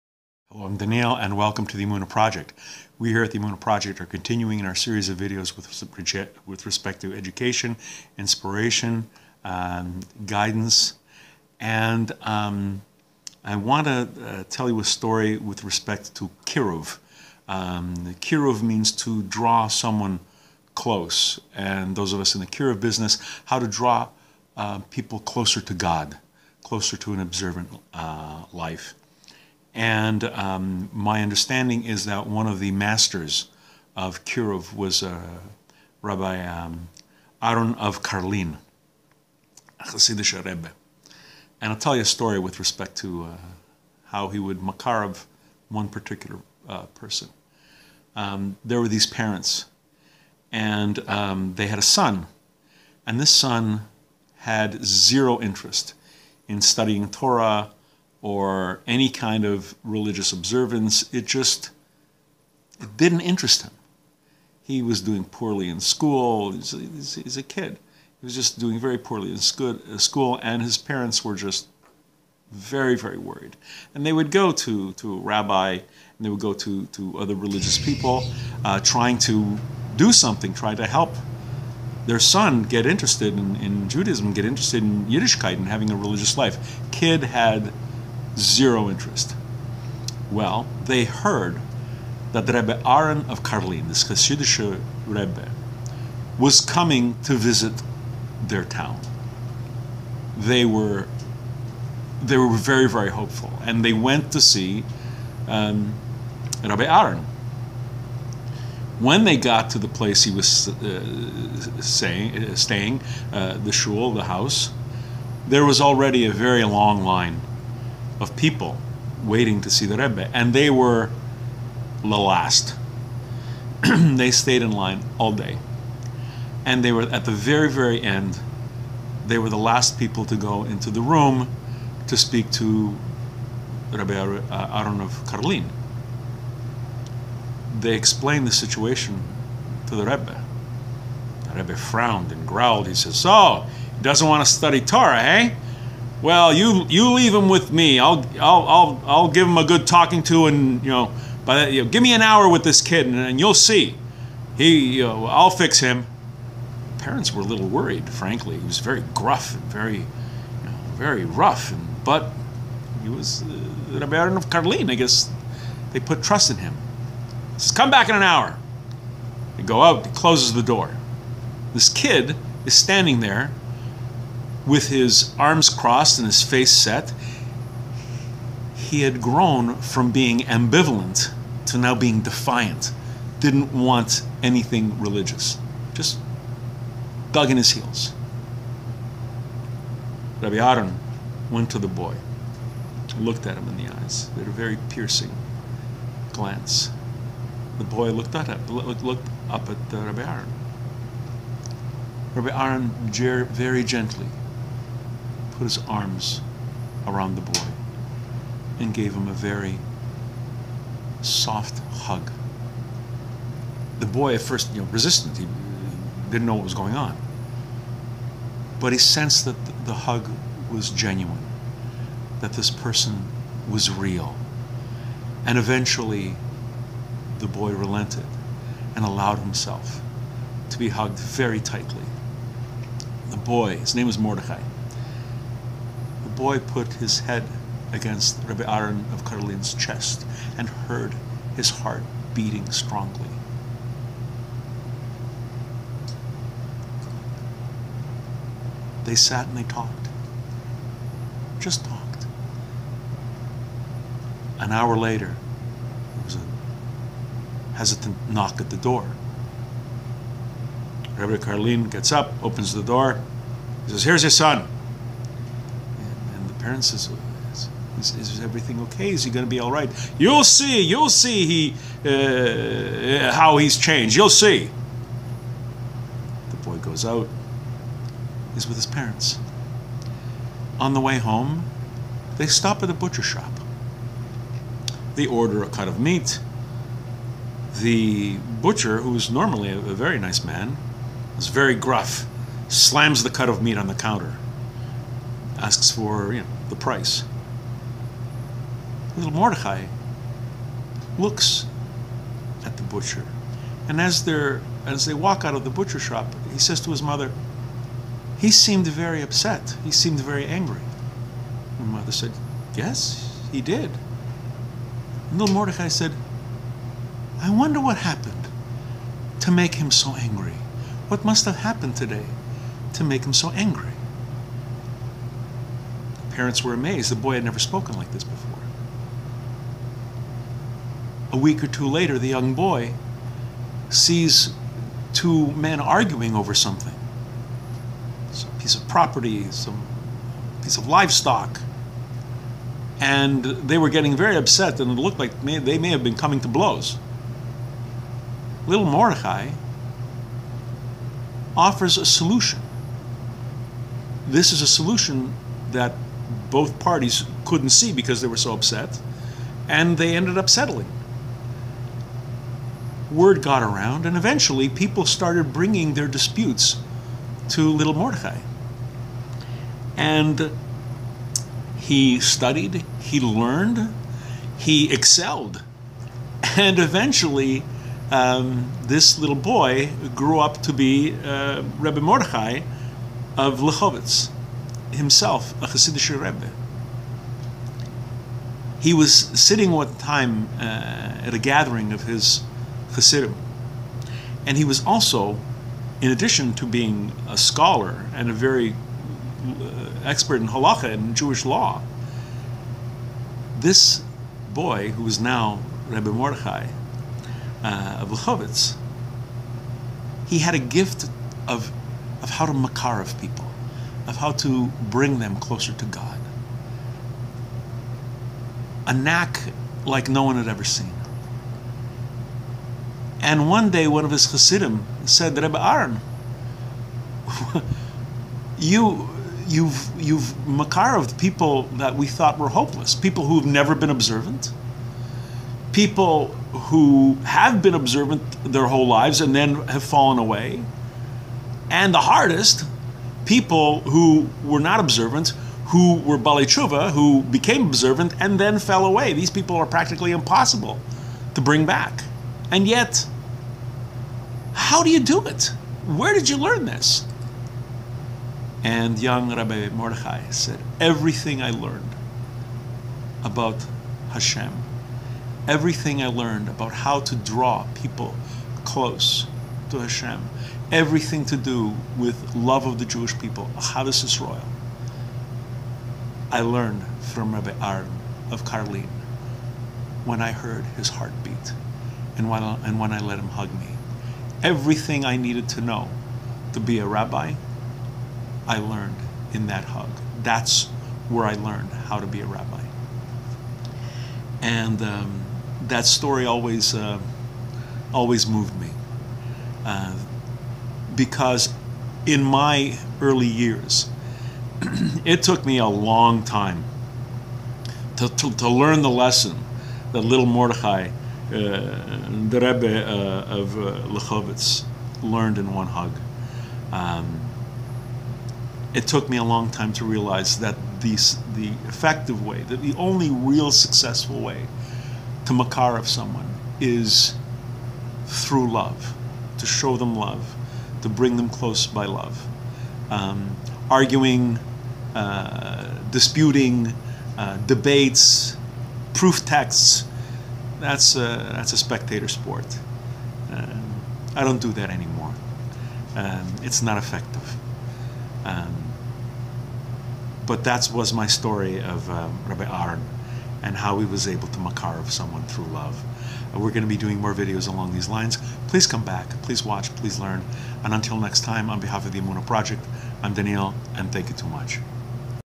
R' Aaron of Karlin and the 'Conversion' of Mordechai
The Emunah Project. R' Aaron of Karlin. R' Mordechai of Lechovitz. Kiruv. Drawing people close to the Creator.